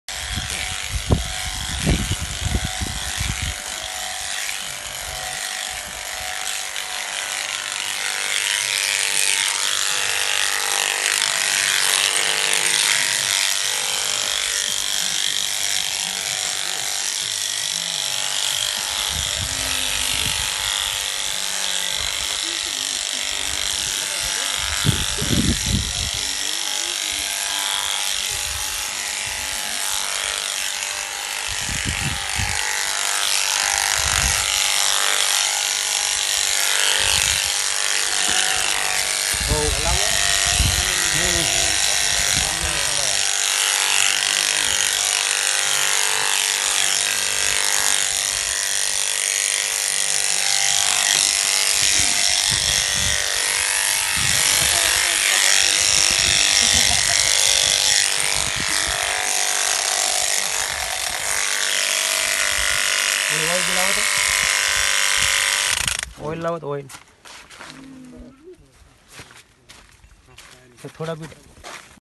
Demonstration of Sheep Hair Shearing Device developed by IIT Delhi at Bhuj Gujarat
This is a demonstration video of Indigenous Sheep Hair Shearing Device developed by IIT Delhi. This demonstration was done at Bhuj, Gujarat in March 2019.